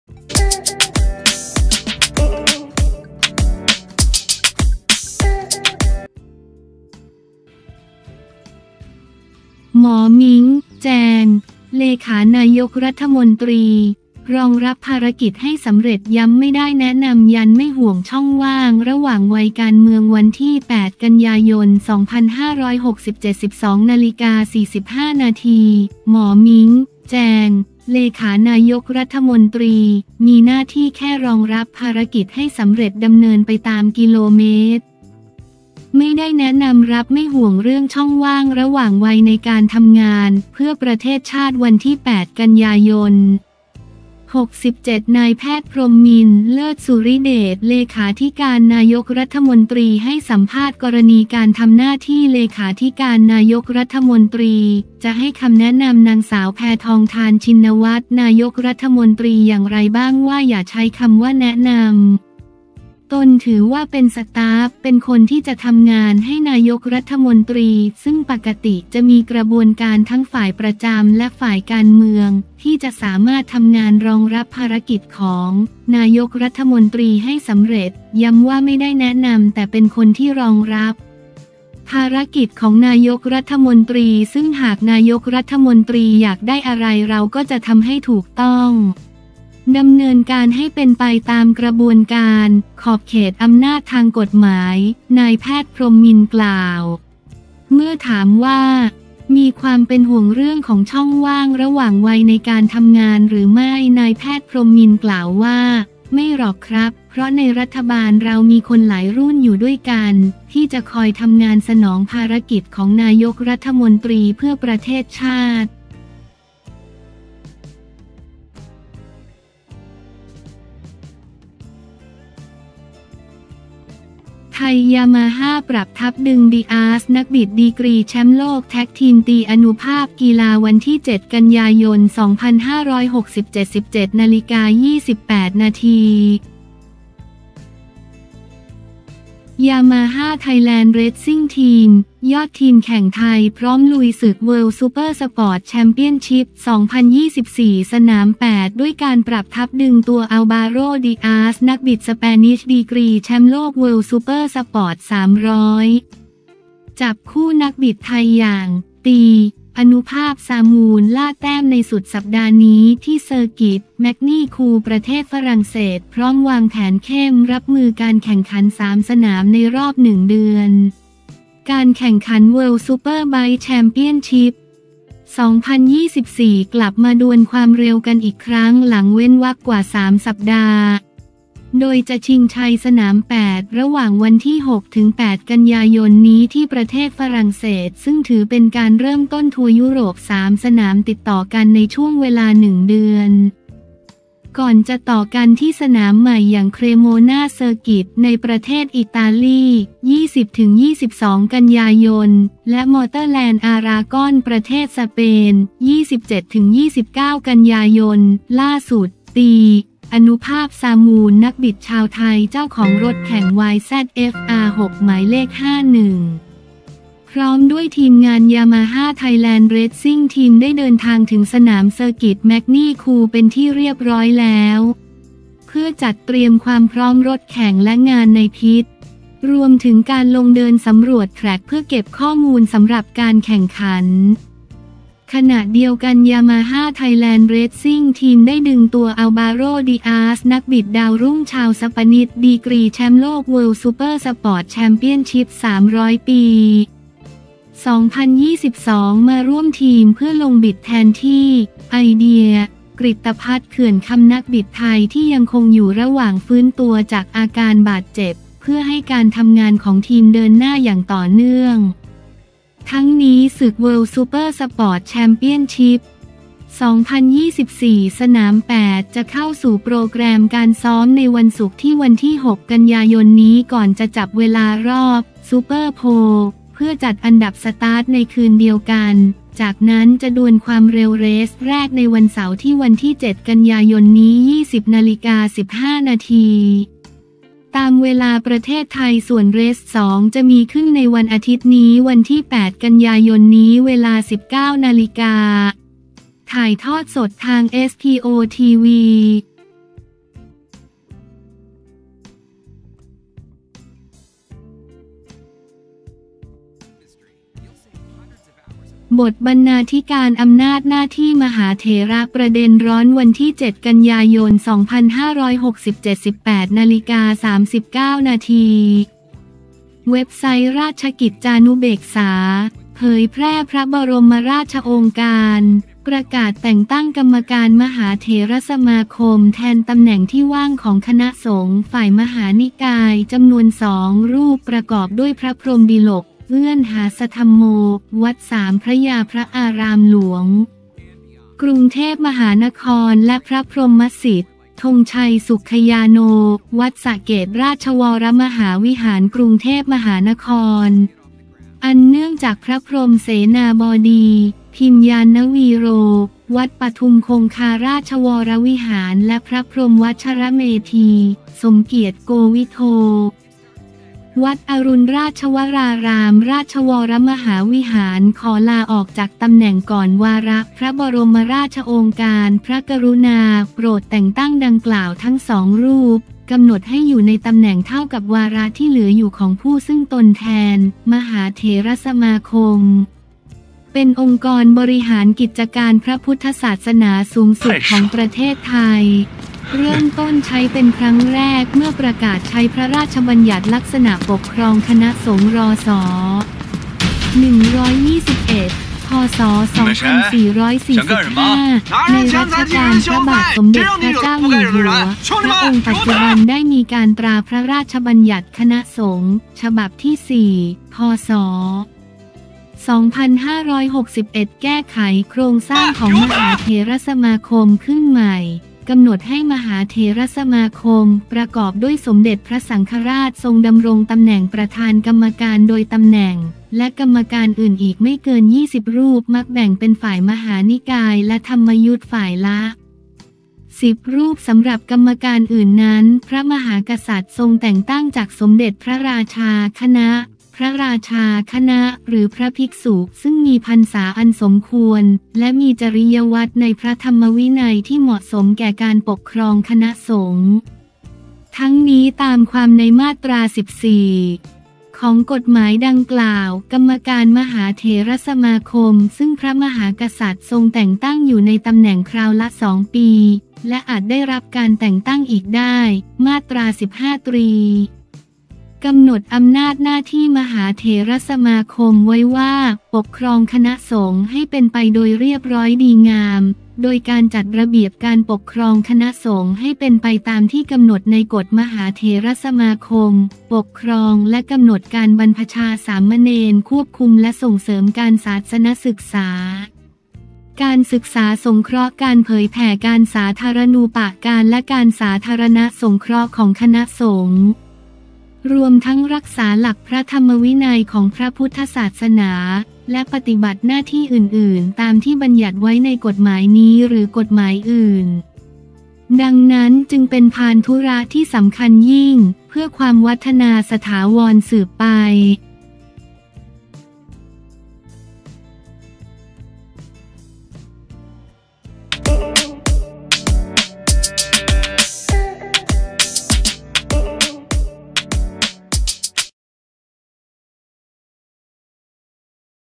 หมอมิ้ง' แจง 'เลขานายกฯ' รองรับภารกิจให้สำเร็จ ย้ำไม่ได้แนะนำ ยันไม่ห่วงช่องว่างระหว่างวัย
8 ก.ย. 2567
12:45 น.
‘หมอมิ้ง’ แจง ‘เลขานายกฯ’ มีหน้าที่แค่รองรับภารกิจให้สำเร็จ-ดำเนินไปตาม กม. ไม่ได้แนะนำ รับ ไม่ห่วงเรื่องช่องว่างระหว่างวัยในการทำงาน เพื่อประเทศชาติ
8 ก.ย. 67 – นพ.พรหมินทร์ เลิศสุริย์เดช เลขาธิการนายกรัฐมนตรี ให้สัมภาษณ์กรณี การทำหน้าที่เลขาธิการนายกรัฐมนตรี จะให้คำแนะนำ น.ส.แพทองธาร ชินวัตร นายกรัฐมนตรี อย่างไรบ้างว่า
อย่าใช้คำว่า แนะนำ ตนถือว่าเป็นสต๊าฟ เป็นคนที่จะทำงานให้ นายกรัฐมนตรี ซึ่งปกติจะมีกระบวนการทั้งฝ่ายประจำและฝ่ายการเมือง ที่จะสามารถทำงานรองรับภารกิจของ นายกรัฐมนตรี ให้สำเร็จ

“ย้ำว่า ไม่ได้แนะนำ แต่เป็นคนที่รองรับ ภารกิจของ นายกรัฐมนตรี ซึ่งหาก นายกรัฐมนตรี อยากได้อะไร เราก็จะทำให้ถูกต้อง ดำเนินการให้เป็นไปตามกระบวนการ ขอบเขตอำนาจทางกฎหมาย” นพ.พรหมินทร์ กล่าว

เมื่อถามว่า มีความเป็นห่วงเรื่องของช่องว่างระหว่างวัยในการทำงานหรือไม่ นพ.พรหมินทร์ กล่าวว่า “ไม่หรอกครับ เพราะในรัฐบาลเรามีคนหลายรุ่นอยู่ด้วยกัน ที่จะคอยทำงานสนองภารกิจของ นายกรัฐมนตรี เพื่อประเทศชาติ